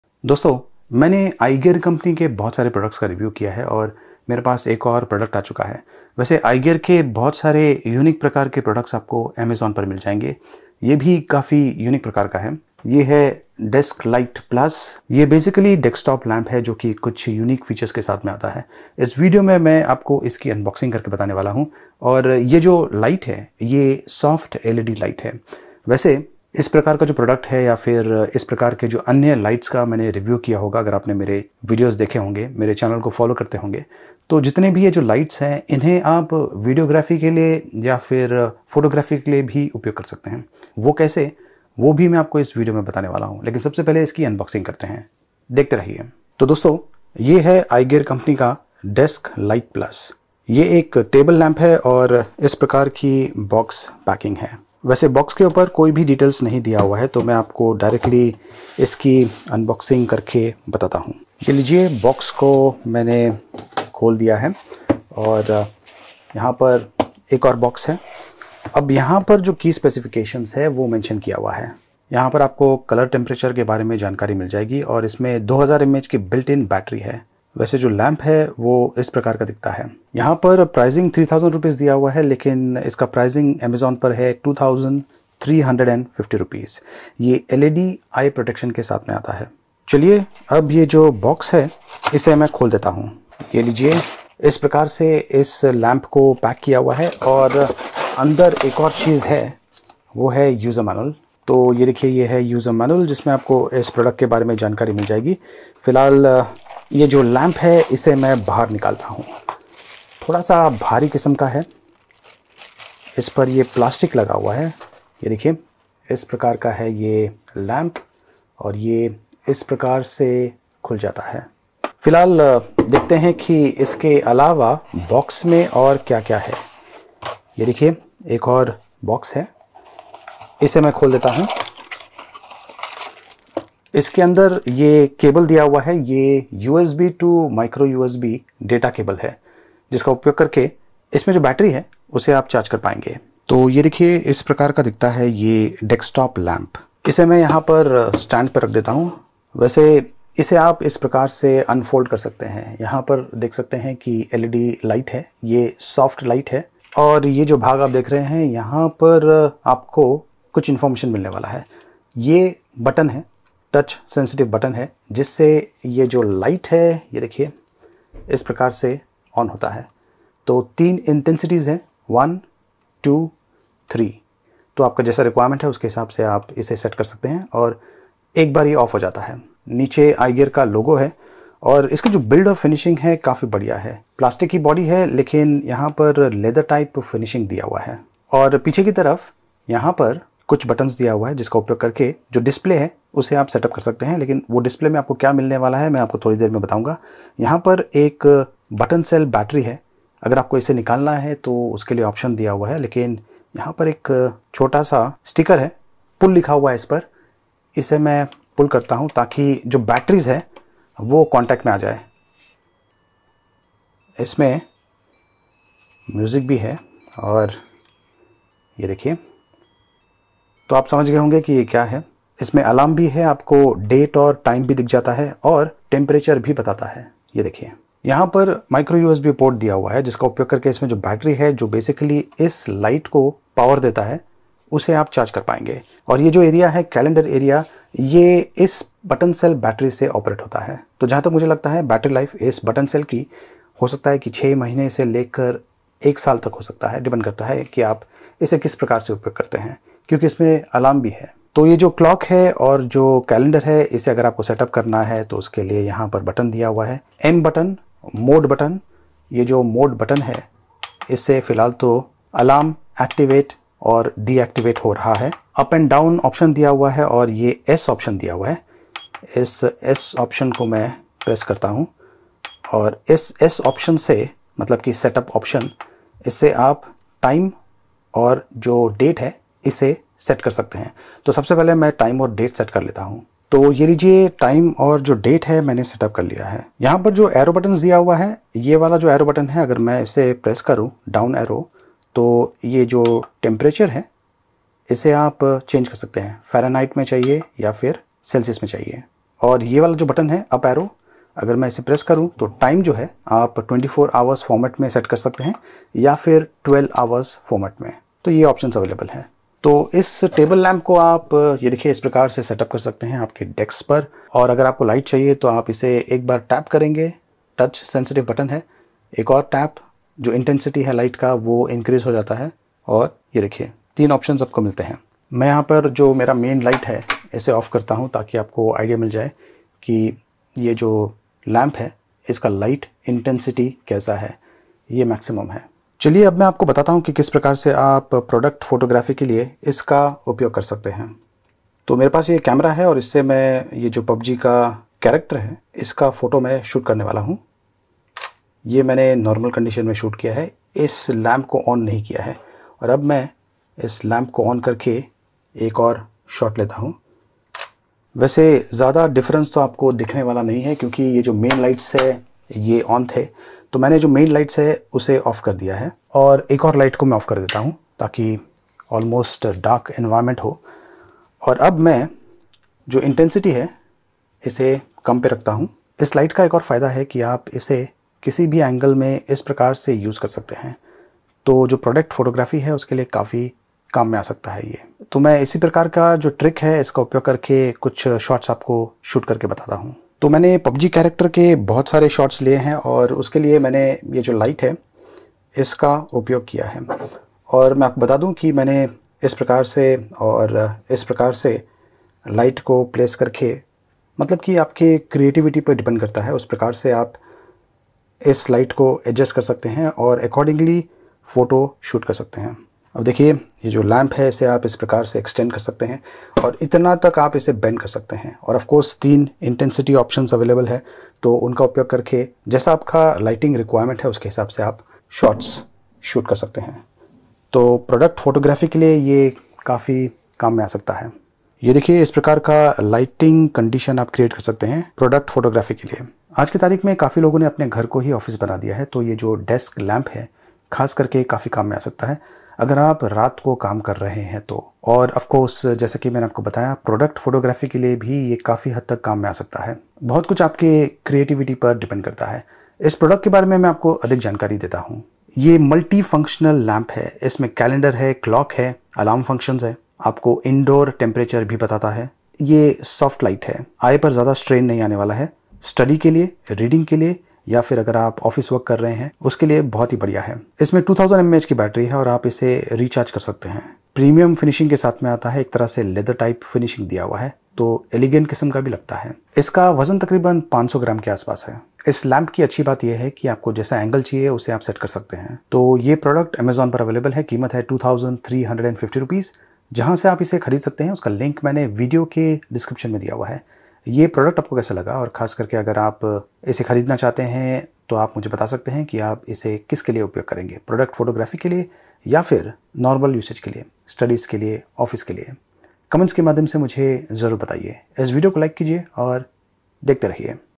iGear Desklite+ Table Led Office Desk Lamp (can use it for video / photos too)
This is iGear Desklite+ lede office desk lamp - students and office users can use this but apart from this it can also be used for product photography.